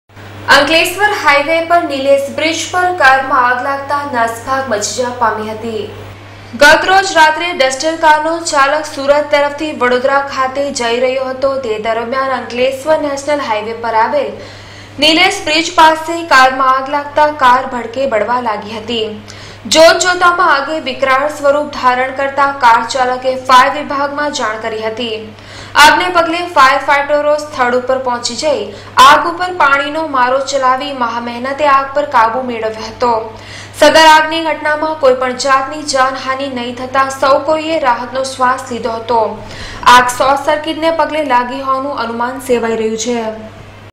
માં ટી.વી. 04-02-2019 અંકલેશ્વર નેશનલ હાઇવે પર નીલેશ બ્રીજ પાસે કારમાં આગ લાગતા
દોડધામ મચી  જવા પામી હતી